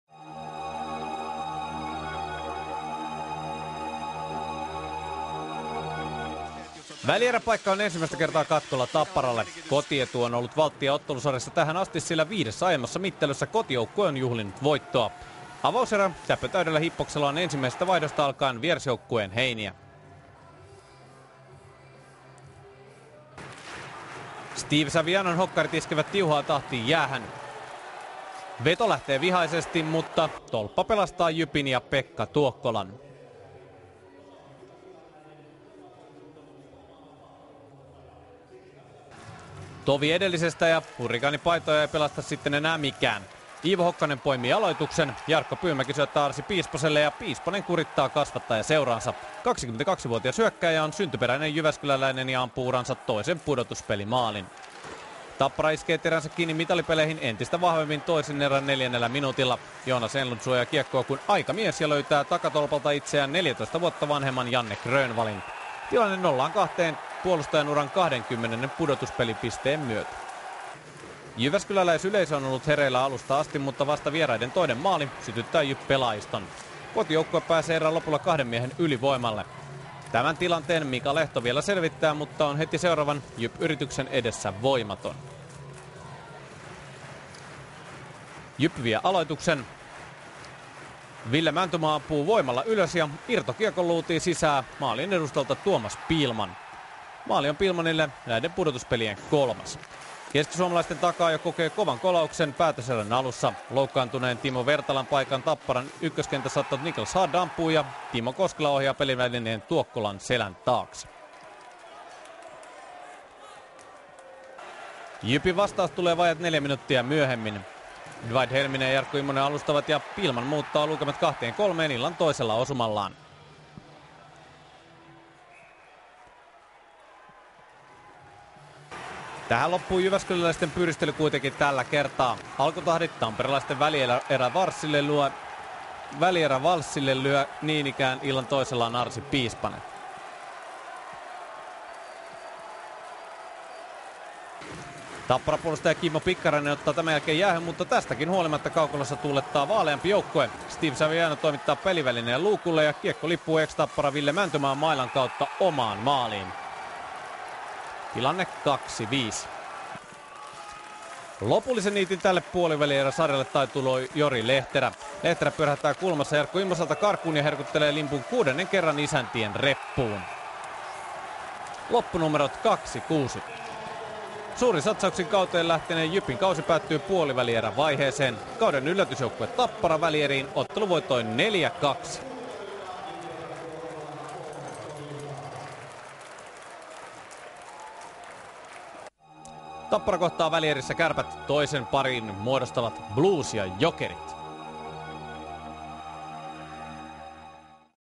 21.3.2008 JYP-Tappara 2-6 (6. puolivälierä)
JYP-Tappara 2-6 (0-1, 1-1, 1-4)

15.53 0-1 Arsi Piispanen (Iivo Hokkanen, Jarkko Pyymäki)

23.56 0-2 Janne Grönvall (Jonas Enlund, Jori Lehterä)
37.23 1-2 Tuomas Pihlman (Ville Mäntymaa, Jarkko Immonen) YV(2)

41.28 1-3 Timo Koskela (Nichlas Hardt, Janne Ojanen)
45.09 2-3 Tuomas Pihlman (Dwight Helminen, Jarkko Immonen)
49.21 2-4 Arsi Piispanen (Iivo Hokkanen, Jarkko Pyymäki)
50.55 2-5 Steve Saviano AV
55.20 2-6 Jori Lehterä (Jonas Enlund, Teemu Nurmi) SR

JYP 3×2 min
Tappara 7×2 min

Pekka Tuokkola 16+7+3=26
Mika Lehto 9+12+6=27

Yleisöä 4500